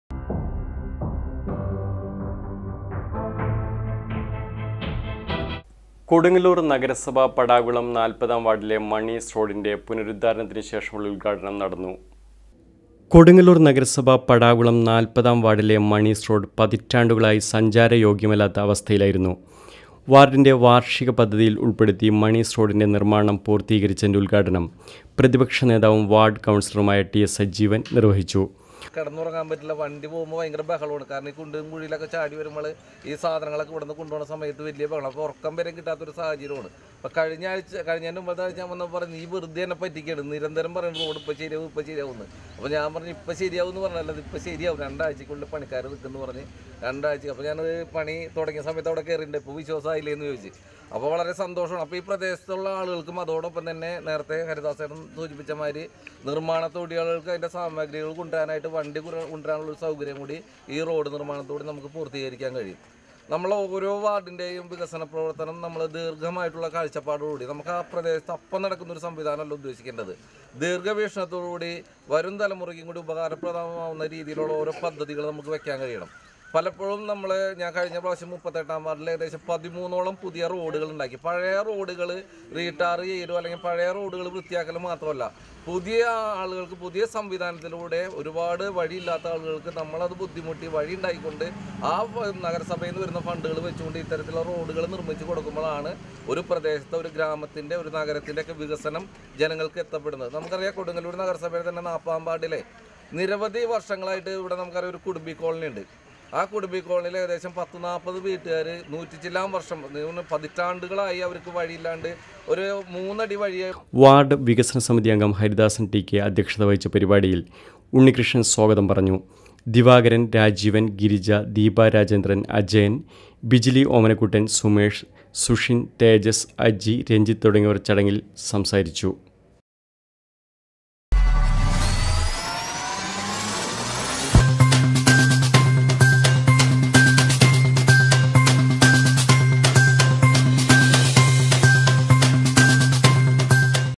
മണീസ് റോഡിൻ്റെ നിർമ്മാണം പൂർത്തികരിച്ചതിന്റെ ഉത്ഘാടനം നടന്നു
പടാകുളം 40-ാം വാർഡിലെ നിർമ്മാണം പൂർത്തികരിച്ച  മണീസ് റോഡിൻ്റെ ഉത്ഘാടനം നടന്നു